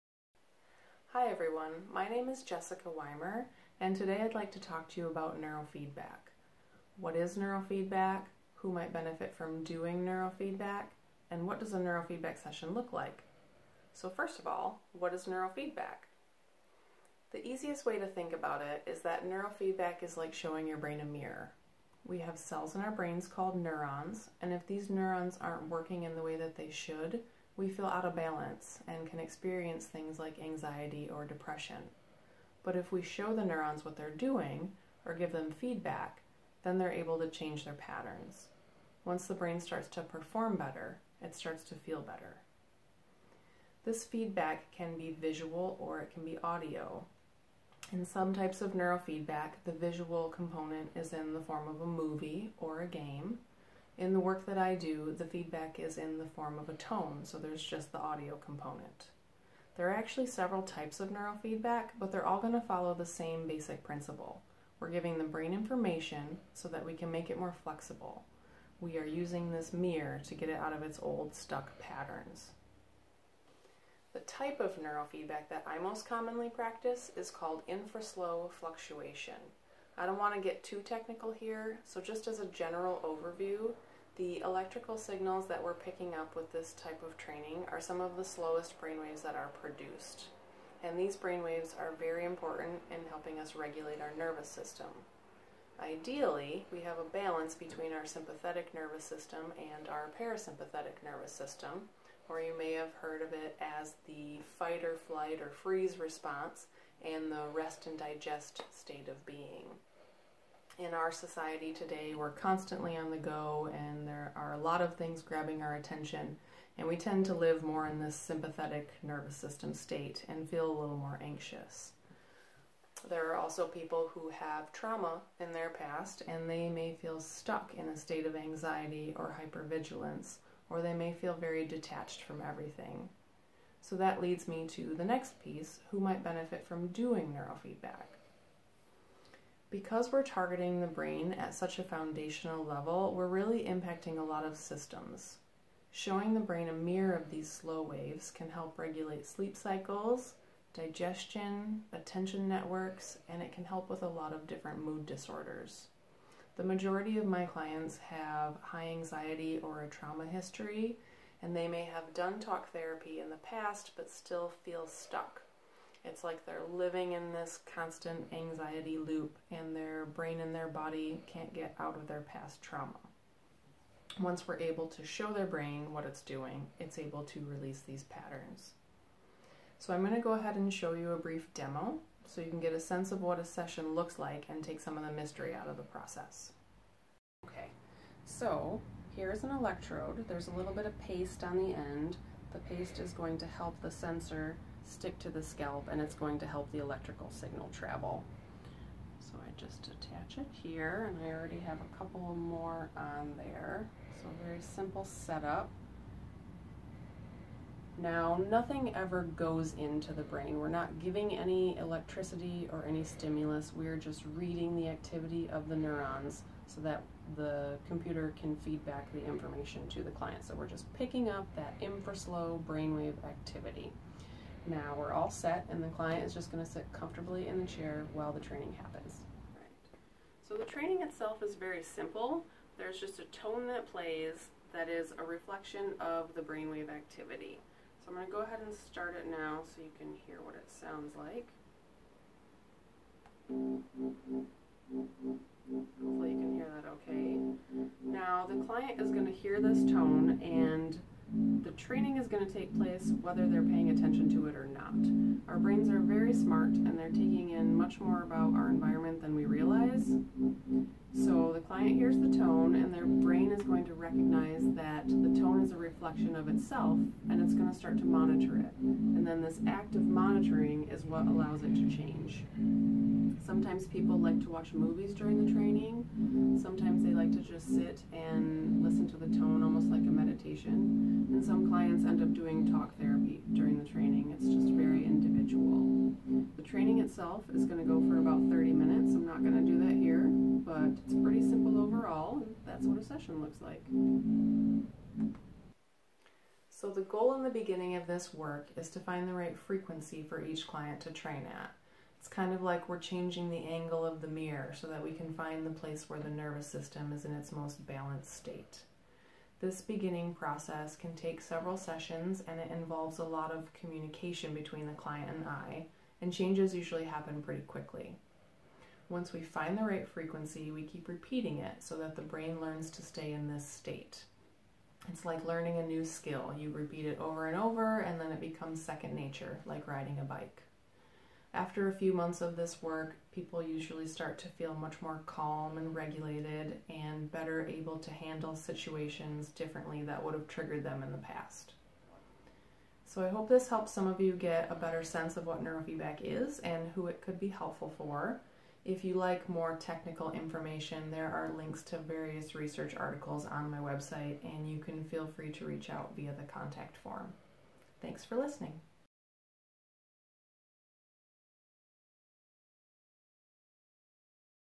What is Neurofeedback? (Infraslow Fluctuation - ISF)
A brief description of neurofeedback, who it may help, and a short demonstration of an ISF neurofeedback session.